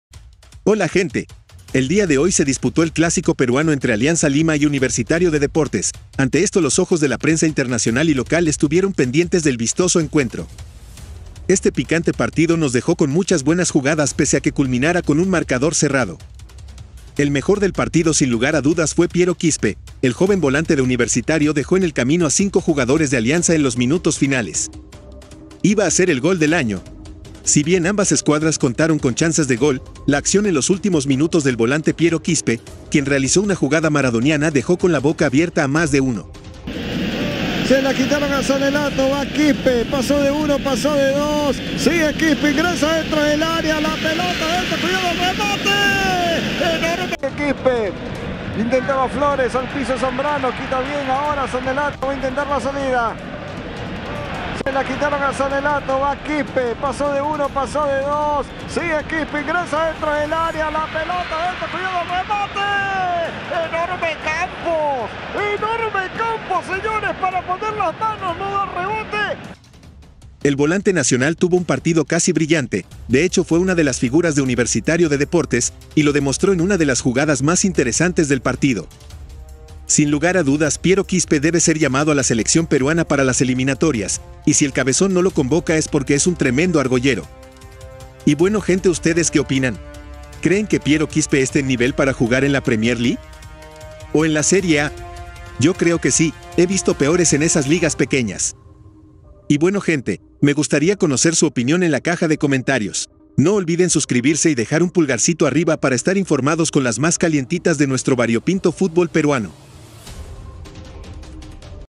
OJITO CON ESTE PERUANO! PRENSA INTERNACIONAL SORPRENDIDA CON LA JUGADA DE PIERO QUISPE ANTE ALIANZA
OJITO CON ESTE PERUANO! PRENSA INTERNACIONAL SORPRENDIDA CON LA JUGADA DE PIERO QUISPE HOY ANTE ALIANZA